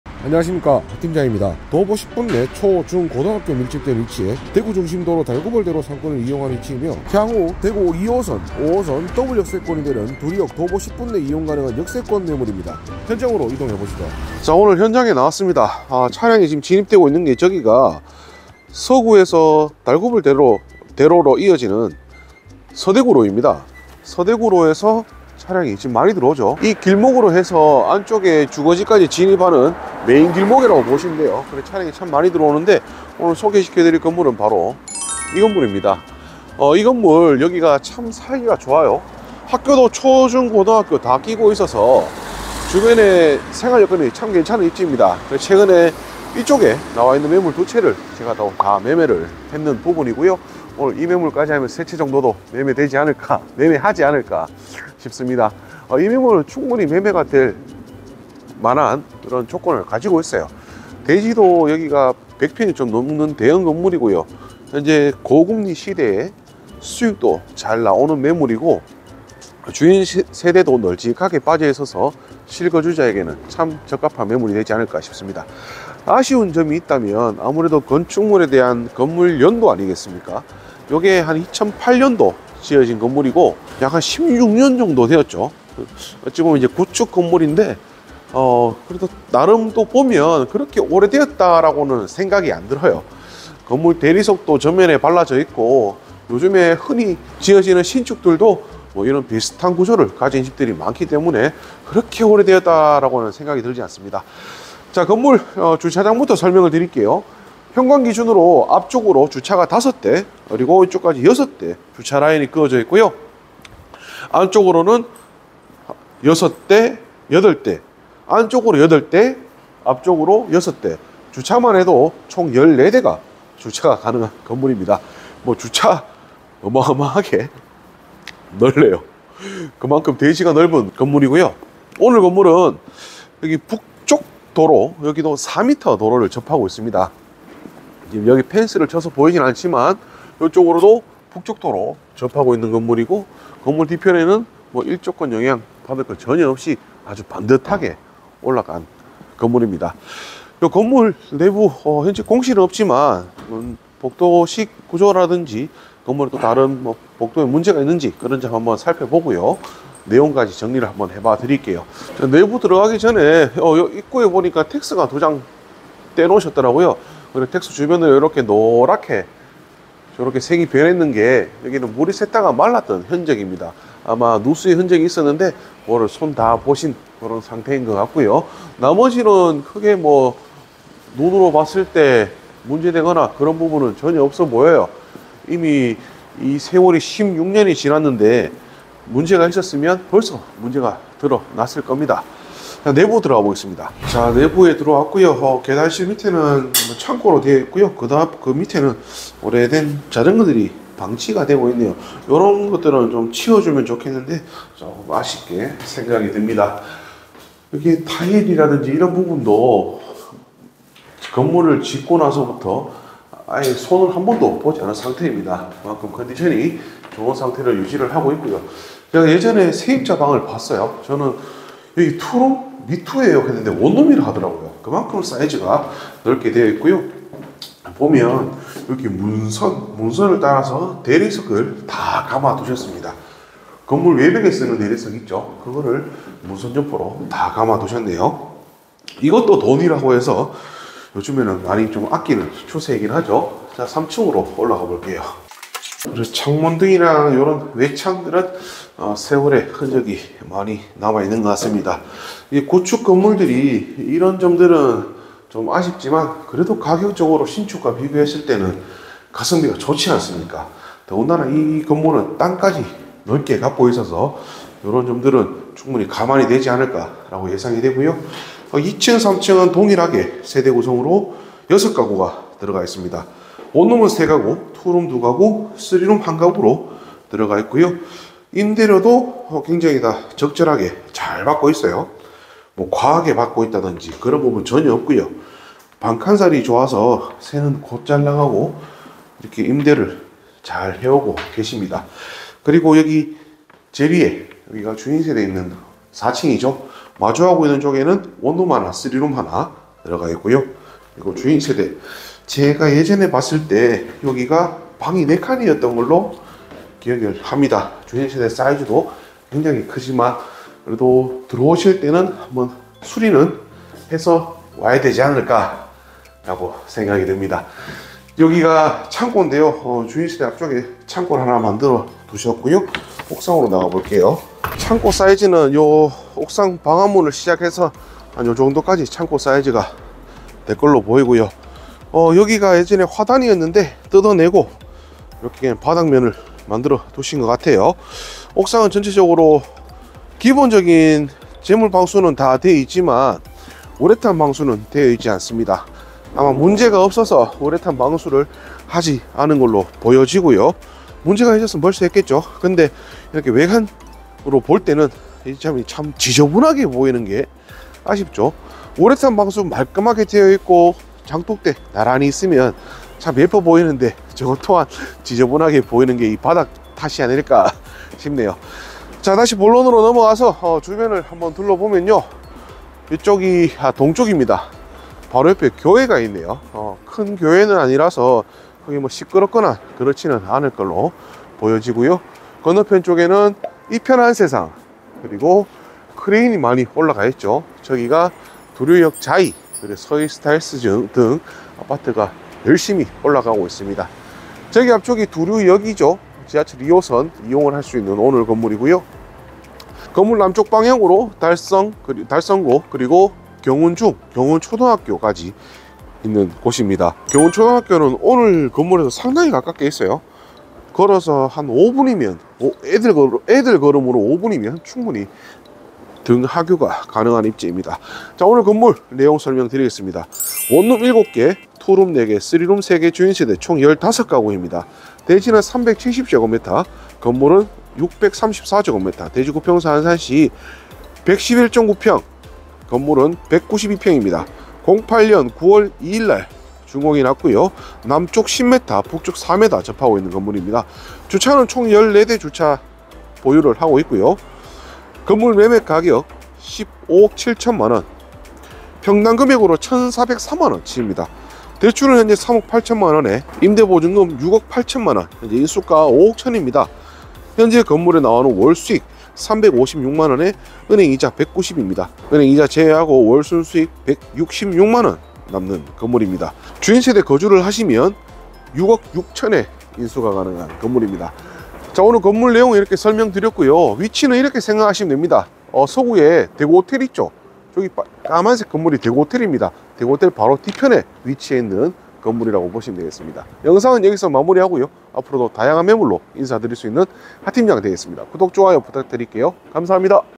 대구원룸매매 2호선 두류역세권 대지111평 가격,위치,수익 세박자 쿵짝!!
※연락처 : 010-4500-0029
※카카오톡 ID : 45000029 (검색)
※해당영상 매물번호: 070223

▶소재지 : 대구광역시 서구 내당동 250-2
▷면적 : 대지 370㎡ (구, 111.9평) / 연면적 634㎡ (구, 192평)

▶거래형태 : 매매
▷거래예정금액 :15억7천만원
▶중개대상물의 종류: 다가구주택

▷사용승인일 : 2008년09월02일
▶총 층수 : 지하 0층 / 지상 4층
▷주차대수 : 14대

▶방향[거실 기준] :남
▷입주가능일 : 등기 이전 후 입주가능
▶방수[근린생활시설 미포함] / 욕실수 : 방 21개 / 욕실 16개

▷관리비[청소비용,승강기 유지비용 등] : 매월 약 200,000원 예상

◆대표:沈民貞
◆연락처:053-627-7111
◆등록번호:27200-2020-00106
◆소속공인중개사 : 하준영